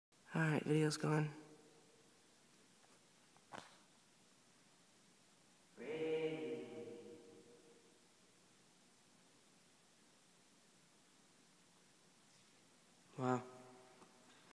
Concord WA "high speed" flight test
The concord walk along glider with relaxing ailerons that make it stay at a fast pace and also stay at a slow pace without adjustments. Flies fast when gliding but slow at walking speed when guided.